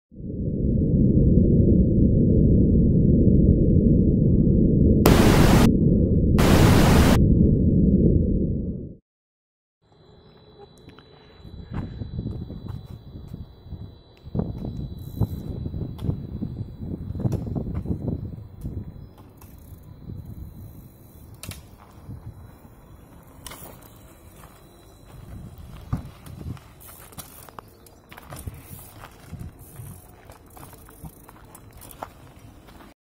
Wyn Masters In the Dam "Barragem do Rio da Mula" Sintra Portugal
Monsas Team was with Wyn Masters at Sintra today and... boy.
"never meet your heroes" is a lame nonsense!
He is an awesome guy and also has an awesome new bike;D
Wyn, I hope you enjoyed your stay and trails!